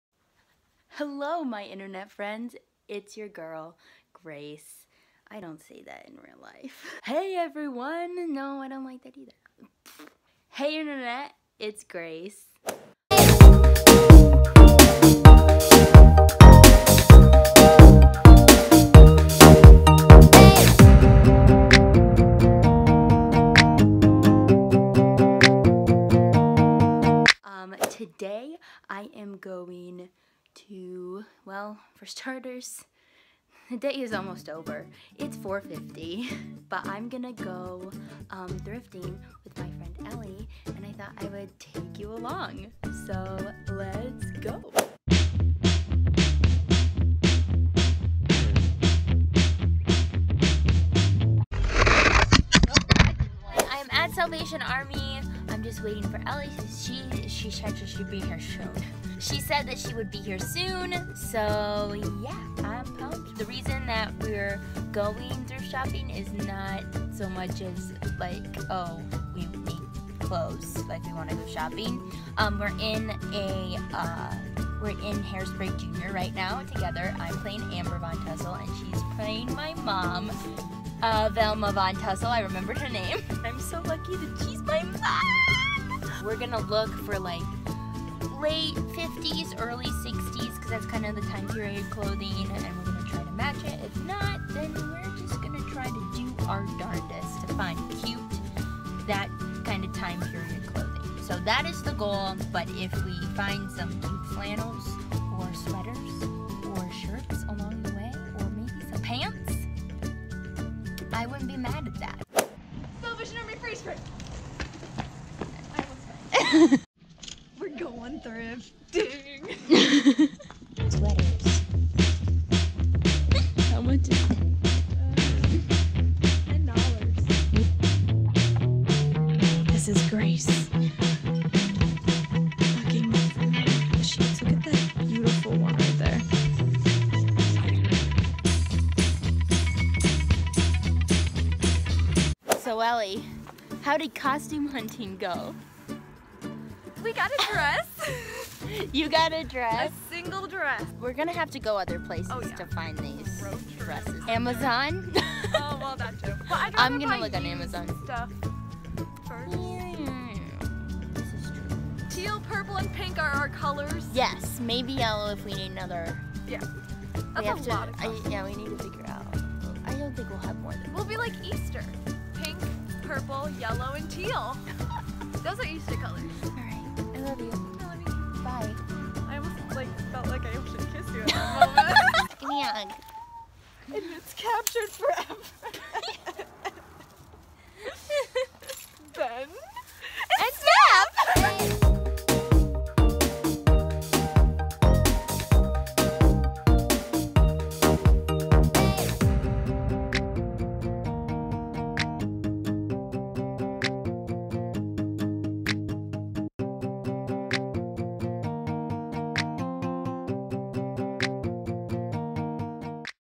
THRIFTING WITH ELLIE|| GRACE MARIE VLOGS
"Two are better than one; because they have a good reward for their labour." -Ecclesiastes 4:9

In this episode of Grace's life Ellie and I go thrifting for costumes for a musical we are in! We have way too much fun together and bring you along for the ride.